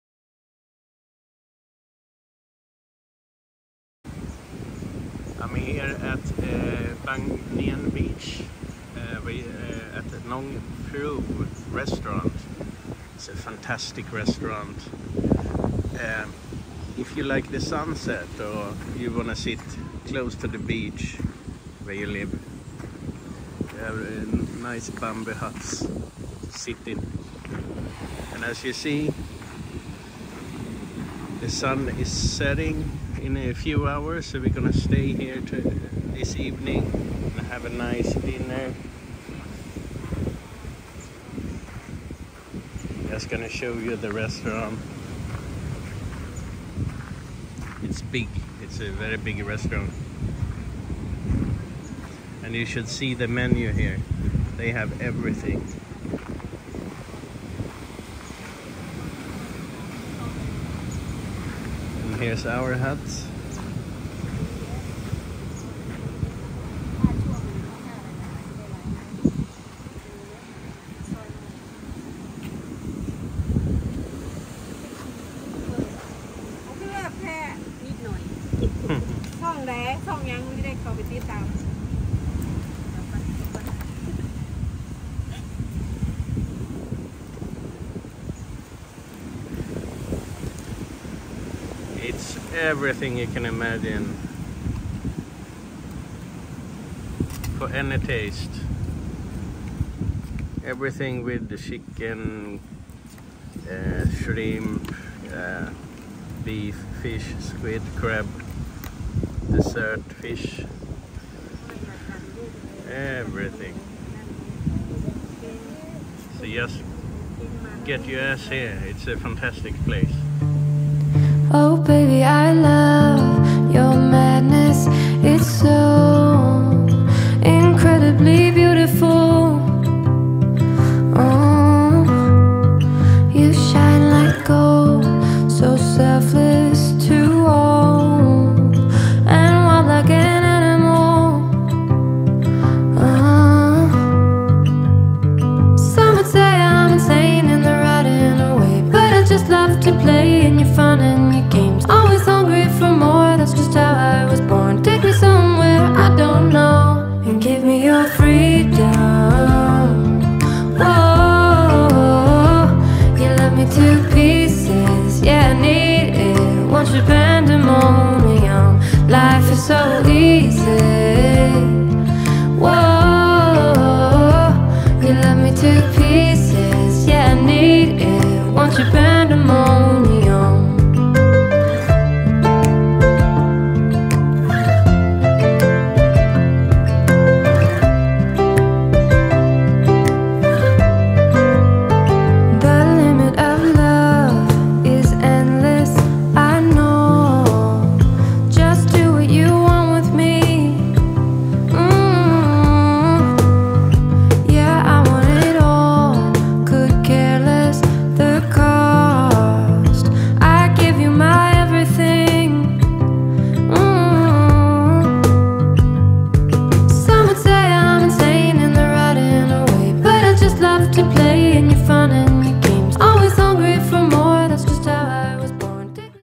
Nong Prew restaurant at Bang Niang Beach Khao Lak, Thailand
IN this video i show you some highlights from our dinner at Nong Prew restaurant at Bang Niang beach which is open now during low season. An amazing menu with everything you can imagine and great food and service.